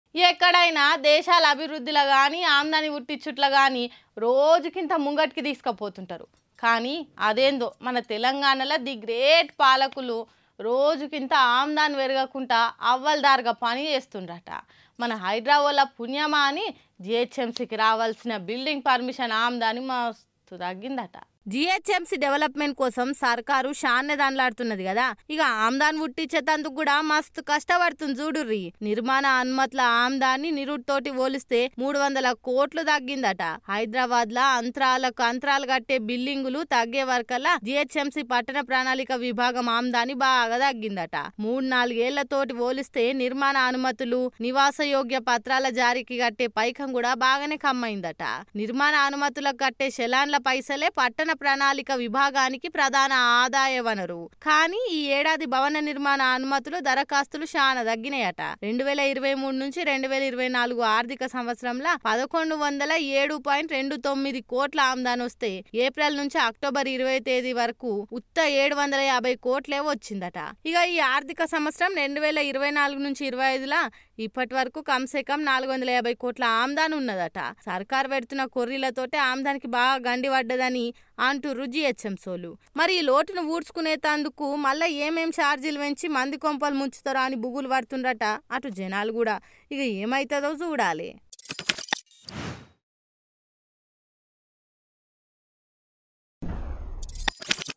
ఇర్రుక్కుపోయిన రేవంత్ సర్కార్..| GHMC Faces Financial Crisis | CM Revanth Reddy | Revenue | Mic Tv
ఇర్రుక్కుపోయిన రేవంత్ సర్కార్..| GHMC Faces Financial Crisis | CM Revanth Reddy | Revenue | Mic Tv News